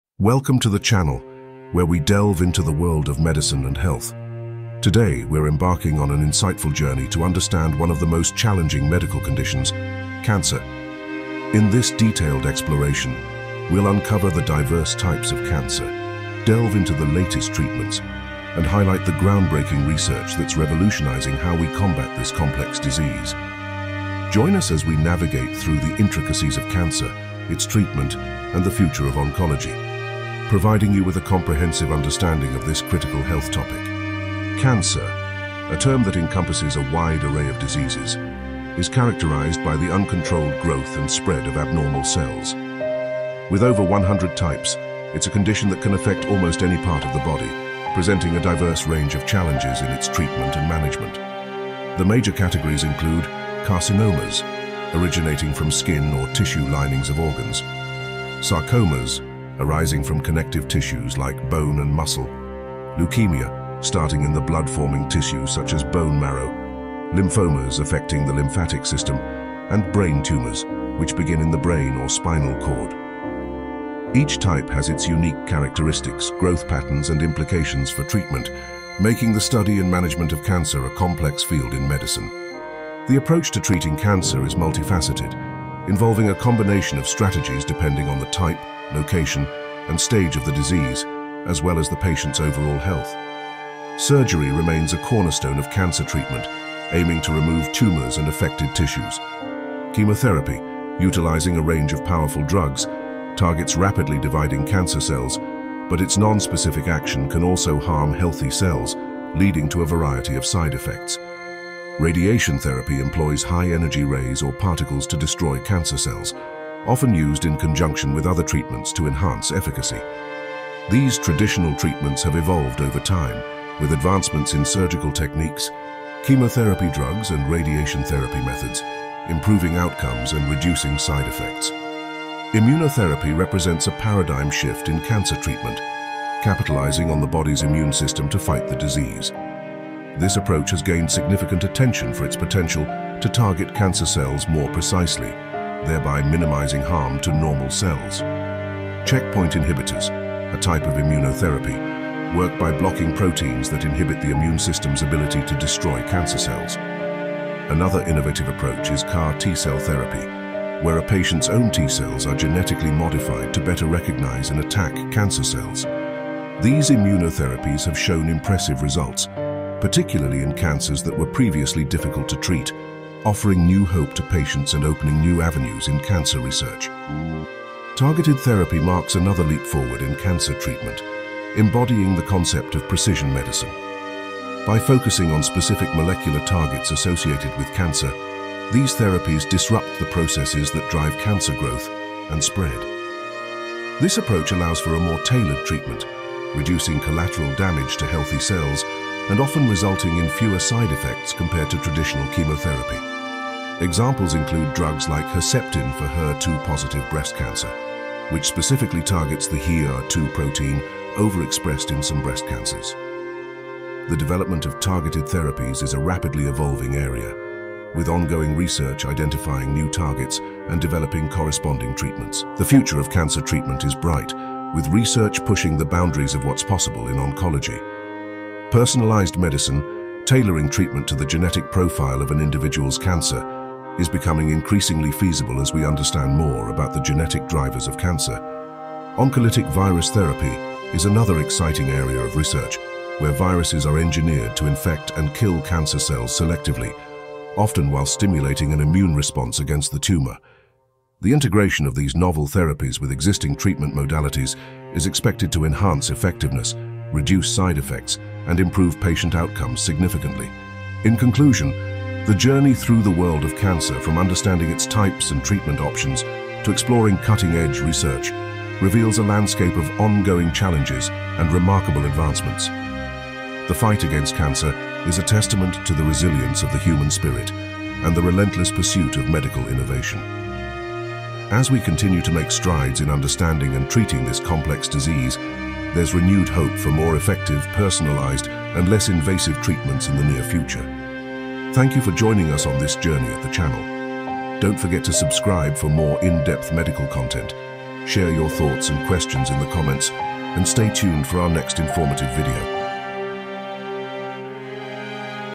Breaking Down Cancer: Types, Treatments, and Research Advances
Description:

Welcome to Captain Camel, your trusted source for medical knowledge and insights. In today's video, we're diving deep into one of the most complex and challenging diseases - cancer. Join us as we explore the multifaceted world of cancer, breaking down its various types, current treatments, and the exciting advancements in research that are paving the way for future therapies.

What You Will Learn:

    Understanding Cancer: An overview of the different types of cancer and how they affect the body.
    Treatment Strategies: We delve into traditional treatments like surgery, chemotherapy, and radiation, and explain when and how they are used.
    Immunotherapy and Targeted Therapy: Learn about these revolutionary approaches that are changing the cancer treatment landscape.
    Research Frontiers: Get insights into cutting-edge research, including personalized medicine and oncolytic virus therapy, which hold promise for more effective and targeted cancer treatments.
    Real-Life Impacts: Hear stories of resilience and hope from patients and medical professionals on the front lines of cancer treatment.

Together, we can navigate the complex world of health and medicine.